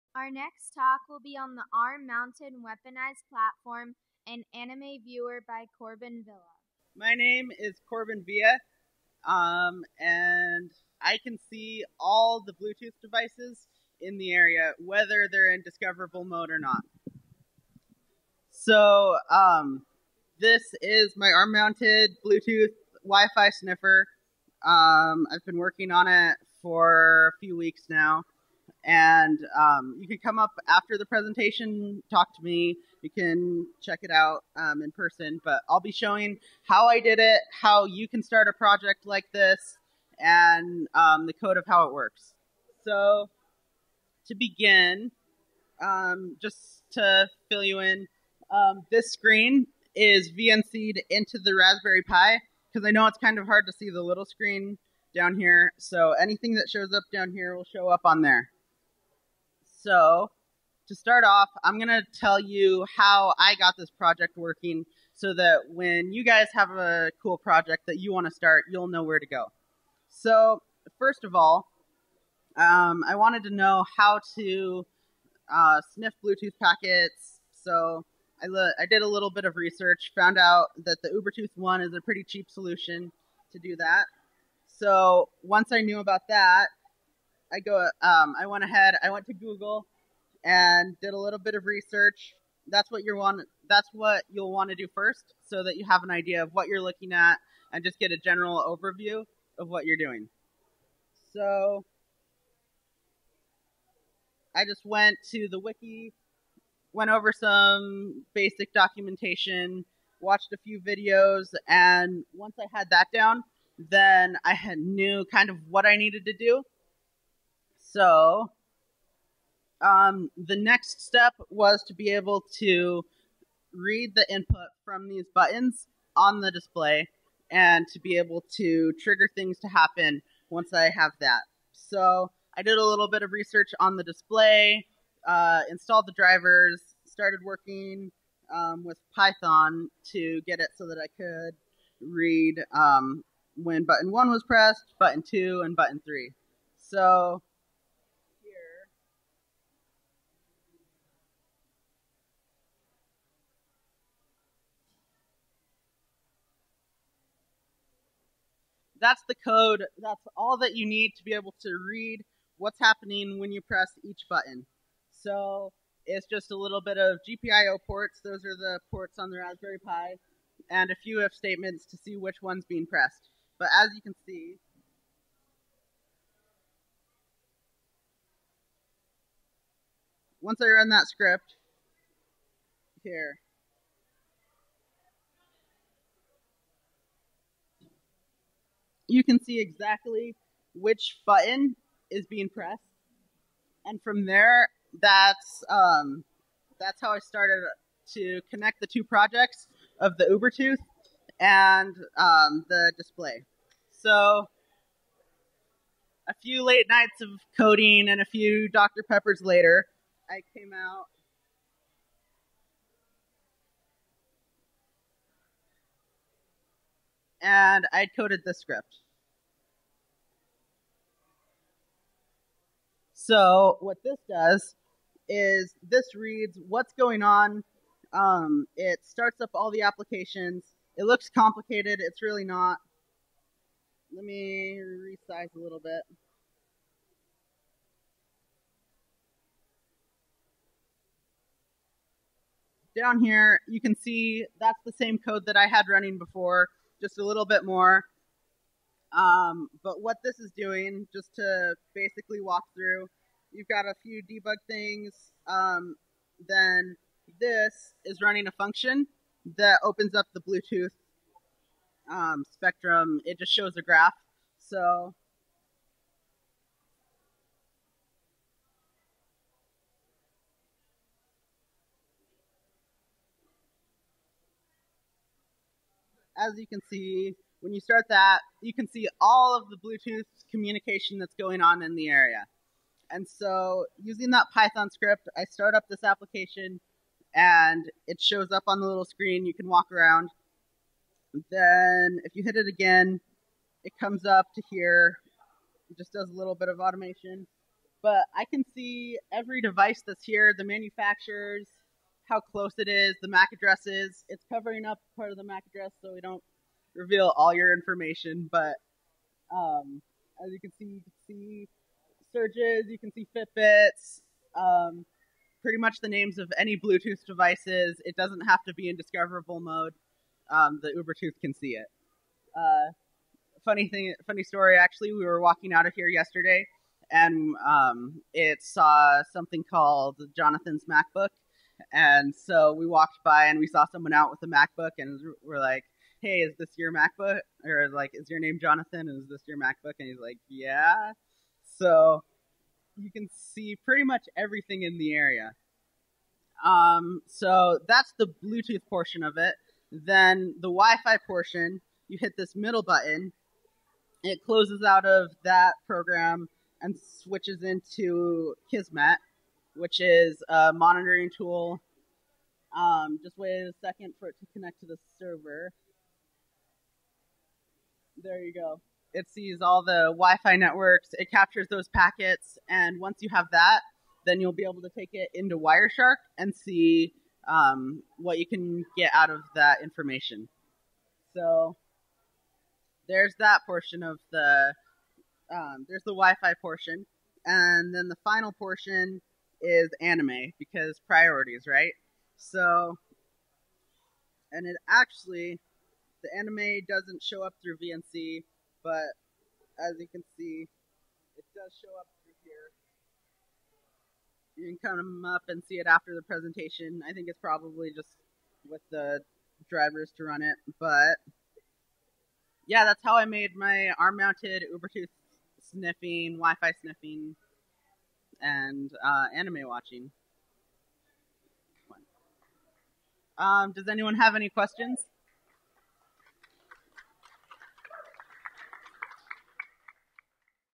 DEF CON 25 Rootz Asylum - Corban Villa - Arm Mounted Weaponized Platform and Anime Viewer
In this talk, we will scan the local area for bluetooth packets with the help of the Ubertooth one, intercept them, and extrapolate as much information as possible, such as tracing mac addresses to find the device type, or brute-forcing encrypted packets to unveil the information you thought to be, “secure.” We will also scan the local area for vulnerable wireless networks using weak encryption methods. Finally, we'll watch hours of anime anywhere with the mobility of a Raspberry Pi for endless entertainment.